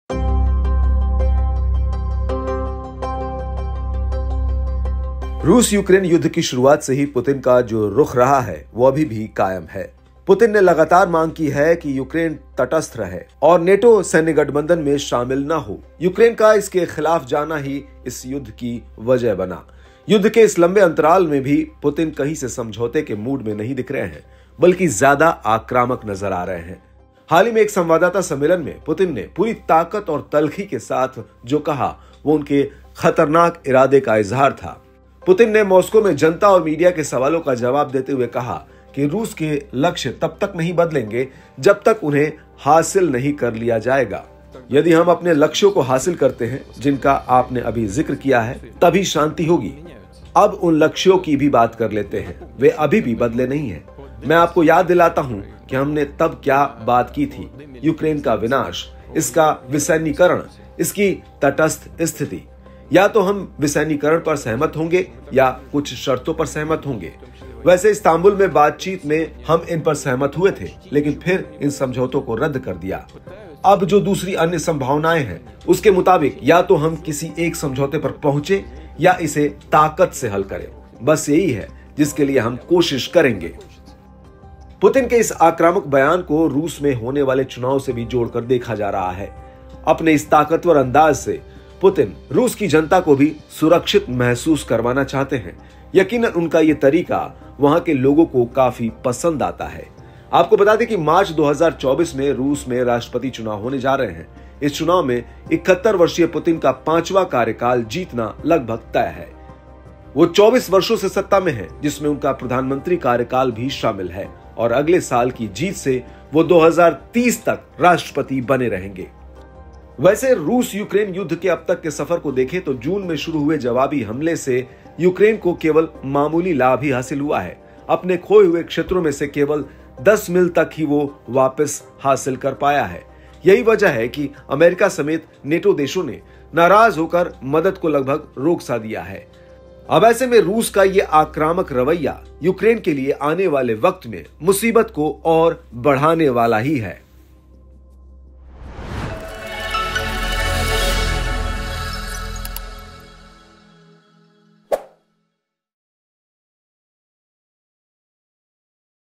Russia Ukraine war: Putin ने कहां Ukraine की बर्बादी तक War जारी रहेगा। Putin I Zelensky IWorld News
Even after this long interval of war, Putin does not seem to be in the mood for compromise from anywhere, but is looking more aggressive. What Putin said with full force and sarcasm at a recent press conference was an expression of his dangerous intent. Putin, answering questions from the public and media in Moscow, said Russia's goals will not change until they are achieved.

युध्द के इस लंबे अंतराल के बाद भी पुतिन कही से समझौते के मूड में नहीं दिख रहे हैं बल्कि ज्यादा आक्रामक नजर आ रहे हैं । हाल ही में एक संवाददाता सम्मेलन में पुतिन ने पूरी ताकत और तल्खी के साथ जो कहा वो उनके खतरनाक इरादे का इजहार था । पुतिन ने मॉस्को में जनता और मीडिया के सवालों का जवाब देते हुए कहा कि रूस के लक्ष्य तब तक नहीं बदलेंगे जब तक  उन्हें हासिल नहीं  कर लिया जाता ।